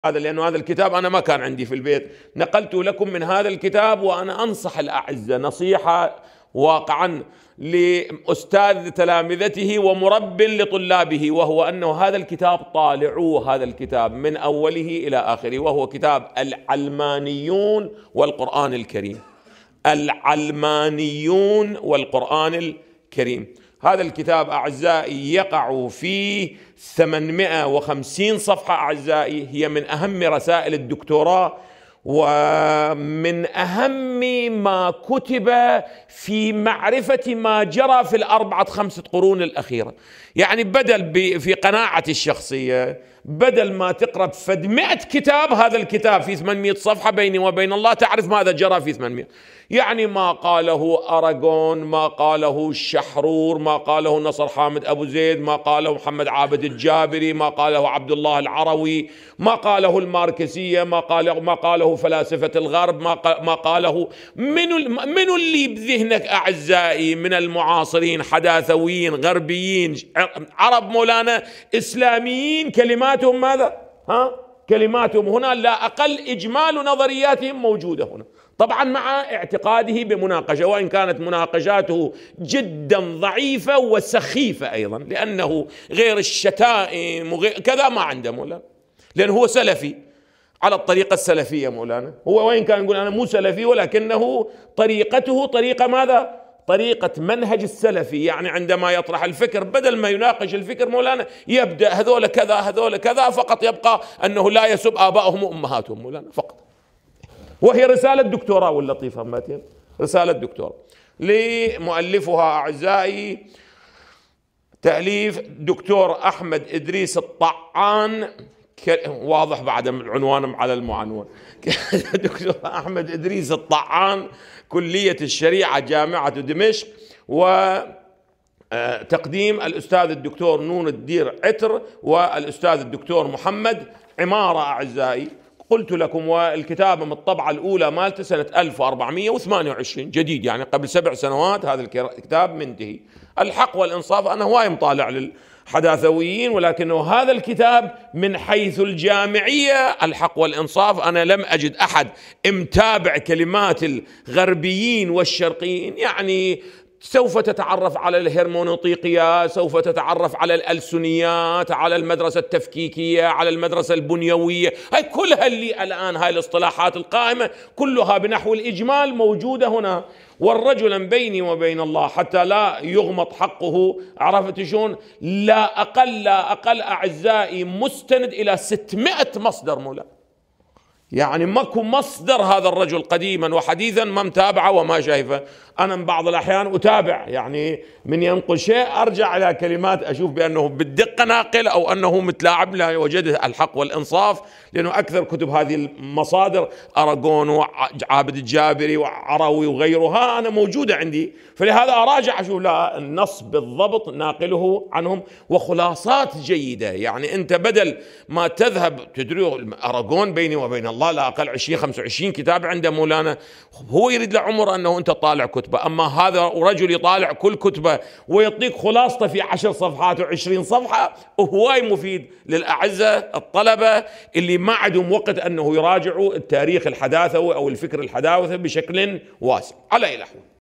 ينصح طلابه بكتاب العلمانيون و القرآن الكريم | السيد كمال الحيدري
هذا الكتاب وانا انصحكم نصيحة واقعاً من استاذ الى تلامذته ولكن مربٍ الى طلابه وهو انه هذا الكتاب طالعوه من اوله الى آخره وهو الكتاب العلمانيون والقرآن الكريم هذا الكتاب يقع في ثمانمائة وخمسين صفحة وهي من اهم الرسائل الدكتورا ومن اهم ما كتب في معرفة ما جرى في اربعة خمسة قرون الاخيرة يعني بدل في قناعة الشخصية بدل ما تقرأ مائة كتاب هذا الكتاب في 800 صفحة هذا الكتاب تعرف ماذا جرى فيها يعني ما قاله ارگون يعني ما قاله شحرور ما قاله نصر حامد ابو زيد ما قاله العابد الجابري ما قاله عبد الله العروي ما قاله الماركسية ما قاله الفلاسفة الغرب ما قاله من الذي في ذهنكم من المعاصرين والحادثويين غربيين عرب اسلاميين كلماتهم هنا لا اقل اجمال نظرياتهم موجودة مع اعتقاده بمناقشة وان كانت مناقشاته جداً ضعيفة وسخيفة بأنه غير الشتائم وكذا ما عنده لانه هو سلفي وعلى الطريقة السلفية وان كان هو يقول انا سلفي ولكنه طريقته طريقة منهج السلفي يعني عندما ينقل الفكر بدل ما يناقش الفكر يبدأ هؤلاء كذا وكذا فقط لا يبقى أن يسب ابائهم وامهاتهم وهي رسالة دكتوراه.

اللطيف أيضاً تأليف دكتور احمد ادريس الطعان كلية الشريعة جامعة دمشق نور الدين عتر واستاذ الدكتور محمد العمارة قلت لكم والكتاب هم الطبعة الاولى لها 1421 جديد يعني قبل سبعة سنوات هذا الكتاب منتهى الحق والانصاف انا كثير طالعت للحادثويين ولكن هذا الكتاب من حيث الجامعية الحق والانصاف انا لم اجد احد متابع كلمات الغربيين والشرقيين يعني سوف تتعرف على هرمونوطيقيات سوف تتعرف على الالسنيات على المدرسة التفكيكية على المدرسة البنوية هذه كلها الاصطلاحات القائمة كلها بنحو الاجمال موجودة هنا والرجل بيني وبين الله حتى لا يغمض حقه لا اقل مستند الى ستمائة مصدر يعني لا يوجد مصدر هذا الرجل قديماً وجديداً لا تابعه انا بعض الاحيان اتابع يعني من انقل شيء ارجع الى كلمات انظر بأنه بالدقة نقل أو انه متلاعب لا وجد الحق والانصاف لانه اكثر كتب هذه المصادر ارگون وعبد الجابر وعروي وغيرها انا موجودة عندي فلهذا انا ارجعه وانظر بانه موجود وخلاصات جيدة لانه انت عندما تذهب تدري بأن ارگون بيني وبين الله عنده 20-25 كتاب لا اقل هو يريد له عمر حتى انت تطالع كتبه أمّا هذا الرجل يطالع كل كتبه ويطيل خلاصته في عشرة صفحات أو عشرين صفحة كثير مفيد للاعزة الطلبة الذين ليس لديهم  الوقت ليراجعوا التاريخ الحداثوي أو الفكر الحداثوي بشكل واسع.